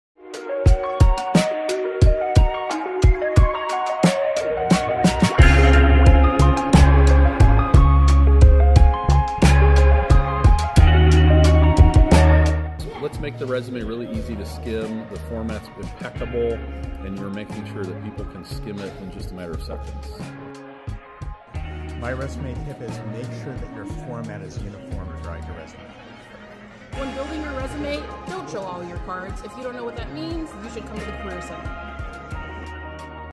Take some careeradvice from the Center for Career and Professional Development! 💼
Looking to up your career development game? 🤝 Here are some resume tips from the Center for Career and Professional Development (CCPD) staff during a workshop in collaboration with Women in Business and Economics!